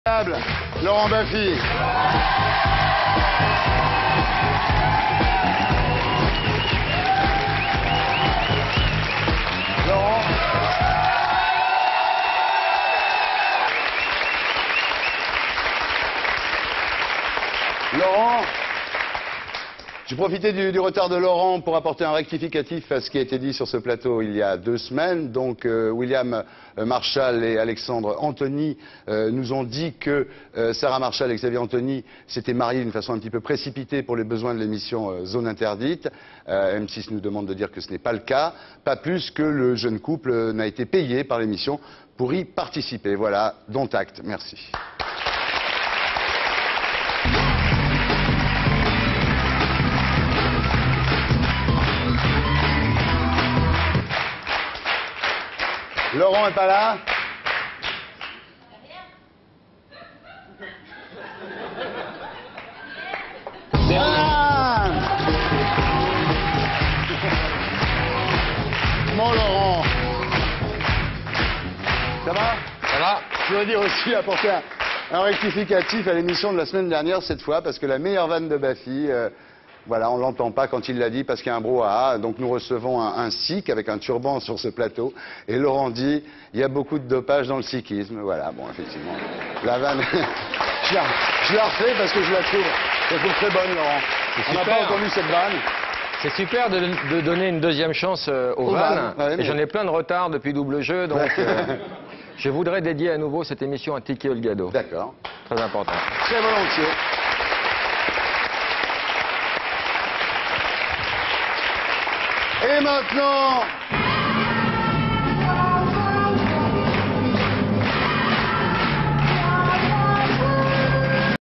Laurent Baffie - Archive INA
Thierry ARDISSON appelle Laurent BAFFIE alors qu'il se cache derrière lui sur le plateau. Il fait un rectificatif sur une précédente émission à la demande de M6.L. BAFFIE demande qu'on dédie l'émission à Ticky HOLGADO. Images d'archive INA